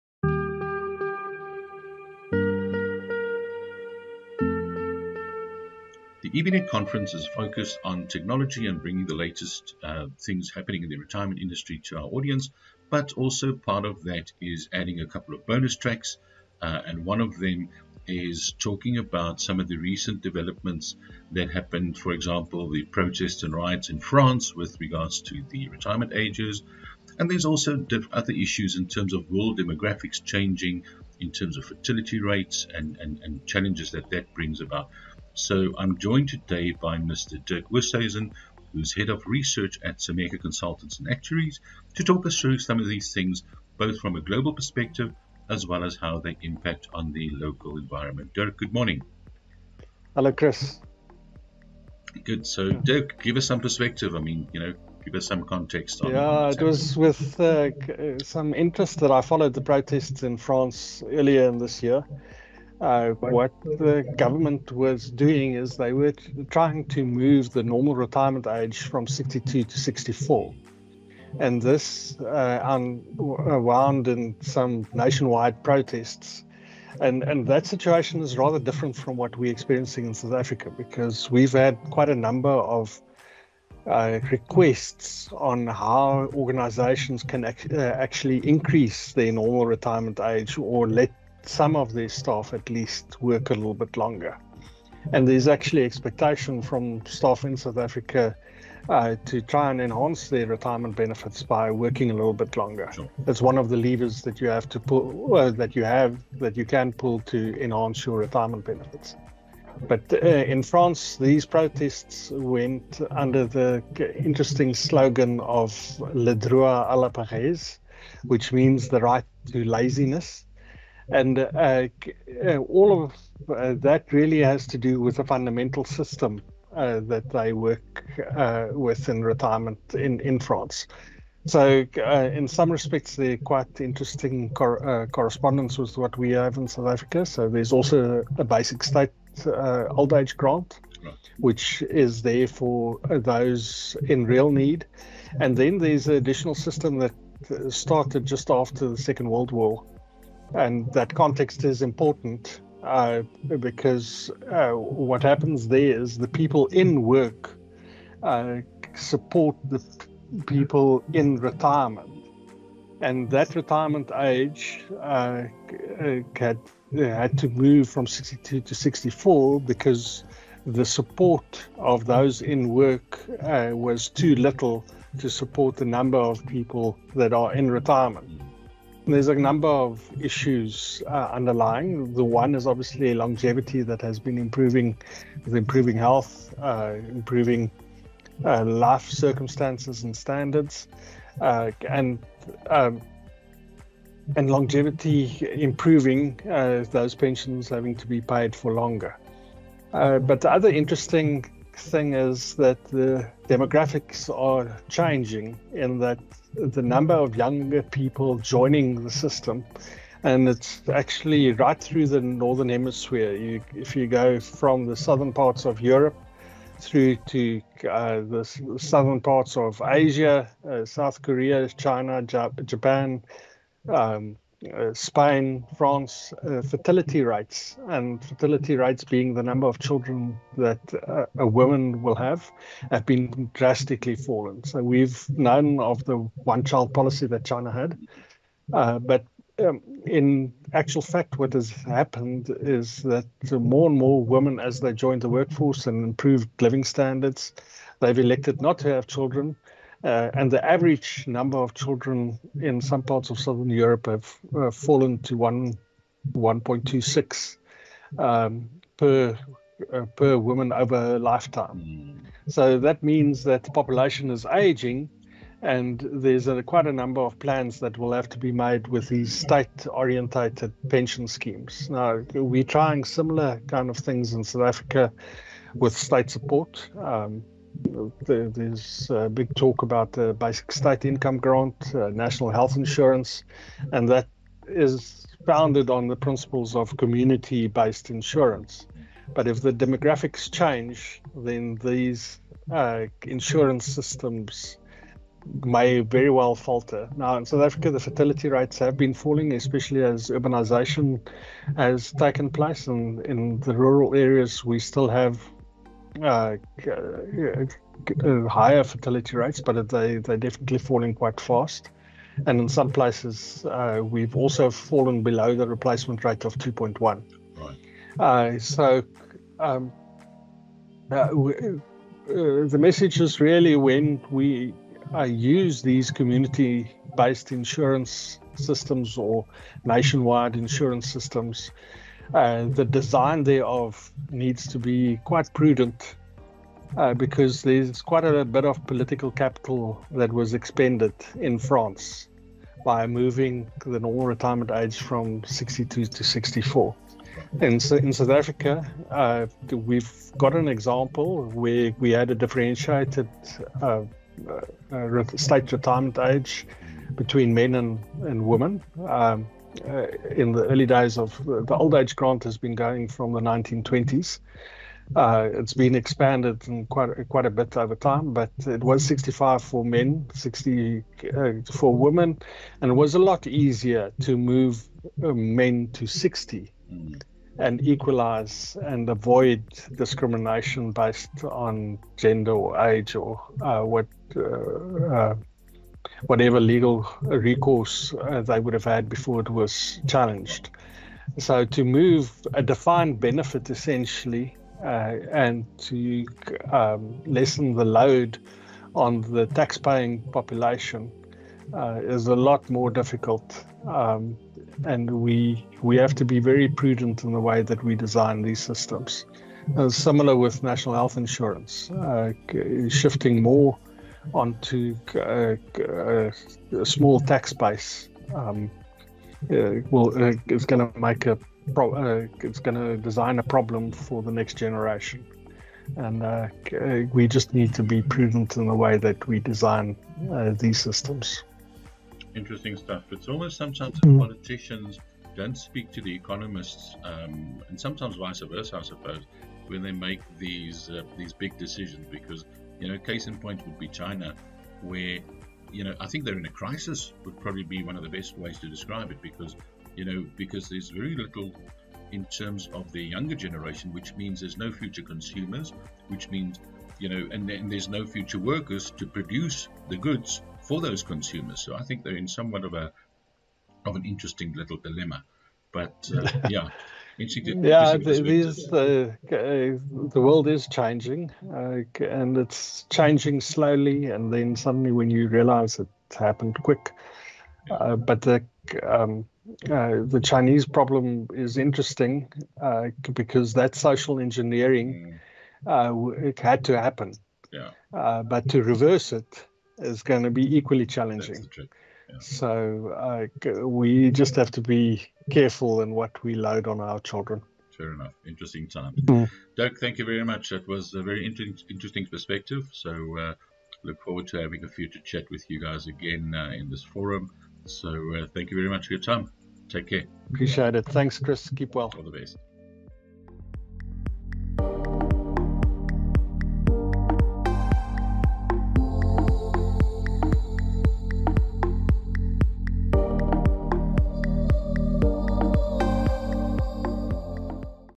Dirk Oosthuizen - Head Research and Development at Simeka Consultants & Actuaries
Global Pension Trends - Increasing retirement age (French example) and Changing fertility rates (Chinese example)

EBnet 3-D Virtual Conference 2023 Interview